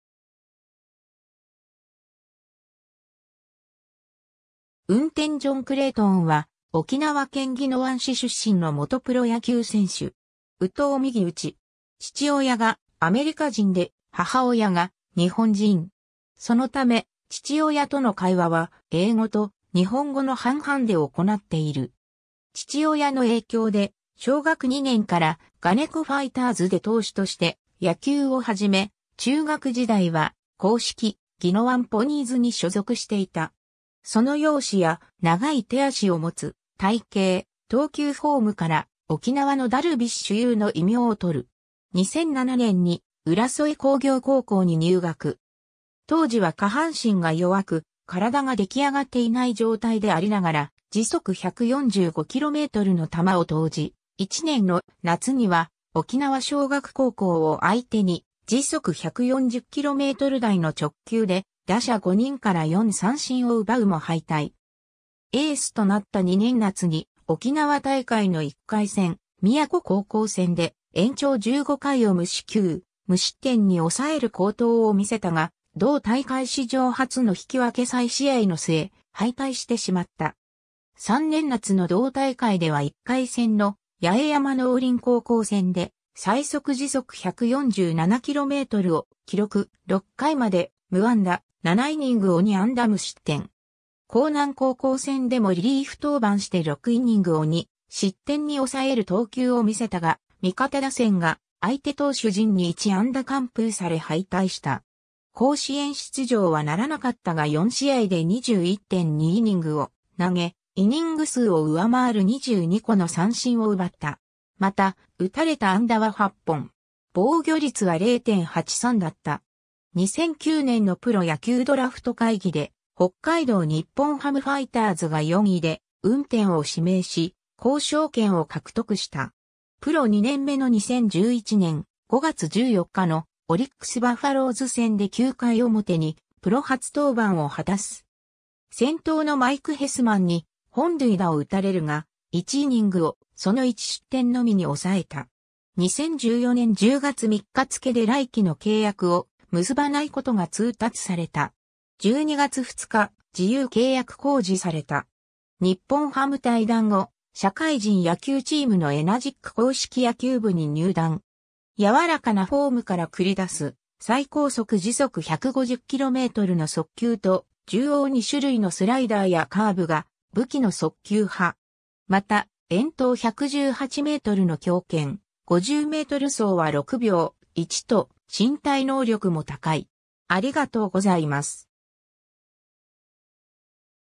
運天ジョン・クレイトン
運天 ジョン・クレイトン（うんてん ジョン クレイトン、1992年3月27日 - ）は、沖縄県宜野湾市出身の元プロ野球選手（投手）。
右投右打。
 父親がアメリカ人で母親が日本人。
そのため、父親との会話は英語と日本語の半々で行っている。
 父親の影響で小学2年から我如古ファイターズで投手として野球を始め、中学時代は硬式・宜野湾ポニーズに所属していた。
その容姿や長い手足を持つ体形、投球フォームから「沖縄のダルビッシュ有」の異名をとる。
 2007年に浦添工業高校に入学。
当時は下半身が弱く体が出来上がっていない状態でありながら145km/hの球を投じ、1年の夏には沖縄尚学高校を相手に140km/h台の直球で打者5人から4三振を奪うも敗退。
エースとなった2年夏に、沖縄大会の1回戦・宮古高校戦で延長15回を無四球・無失点に抑える好投を見せたが、同大会史上初の引き分け再試合の末敗退してしまった。
3年夏の同大会では1回戦の八重山農林高校戦で最速147km/hを記録、6回まで無安打、7イニングを2安打無失点。
興南高校戦でもリリーフ登板して6イニングを2失点に抑える投球を見せたが、味方打線が相手投手陣に1安打完封され敗退した。
甲子園出場はならなかったが4試合で21.2イニングを投げ、イニング数を上回る22個の三振を奪った。
また、打たれた安打は8本。
防御率は0.83だった。
 2009年のプロ野球ドラフト会議で、北海道日本ハムファイターズが4位で運天を指名し、交渉権を獲得した。
 プロ2年目の2011年、5月14日のオリックス・バファローズ戦で9回表にプロ初登板を果たす。
先頭のマイク・ヘスマンに本塁打を打たれるが、1イニングをその1失点のみに抑えた。
 2014年10月3日付けで来季の契約を結ばないことが通達された。
12月2日、自由契約公示された。
 日本ハム退団後、社会人野球チームのエナジック硬式野球部に入団。
 柔らかなフォームから繰り出す、最高速150km/h（2010年ファームで計測）の速球と縦横2種類のスライダーやカーブが武器の速球派。
また遠投118メートルの強肩、50メートル走は6秒1と身体能力も高い。